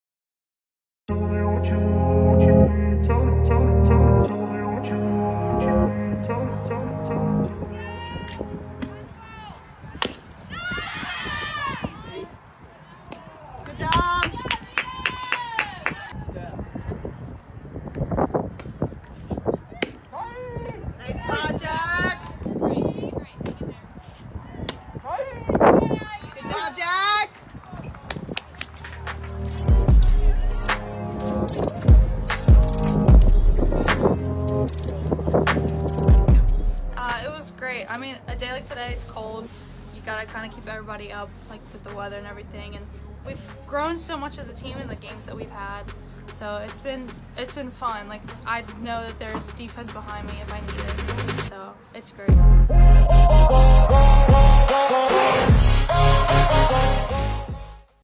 AACC Softball Perfect Game vs Christendom, April 3, 2022
Pitcher Jaclyn Nevins struck out 13 in a perfect game vs Christendom at the AACC Softball Field.